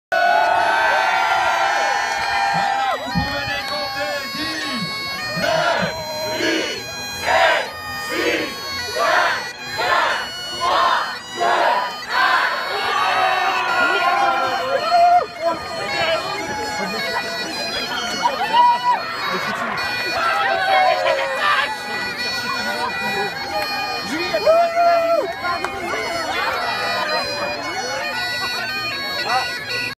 La Boue Fédère 2018 - 3e édition - Coutances
A Coutances (Manche), la Boue Fédère a attiré 2 000 participants, dimanche 24 juin 2018. Au menu : 32 obstacles répartis tout au long des 9 km de ce parcours… particulièrement boueux. Petit aperçu de l'ambiance au moment du départ.